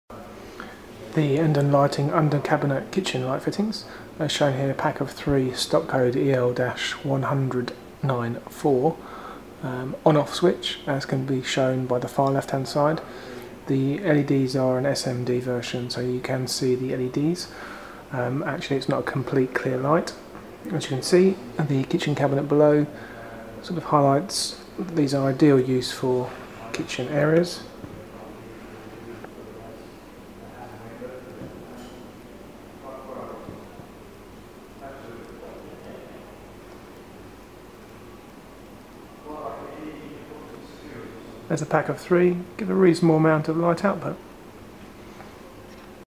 Endon Lighting Under Cabinet Kit in Stainless Steel with LED's - EL-10094
EL-10094 Under Cabinet Kit suitable for Kitchens and Display Lighting - 3 X LED Units with 18 X 0.08w LED's and plug in driver.

    Dimensions (mm): D 125 x P 42 x H 130
    Wattage: 4.3W
    Lamp Holder: LED
    Finish: Stainless Steel
    Bulb: LED included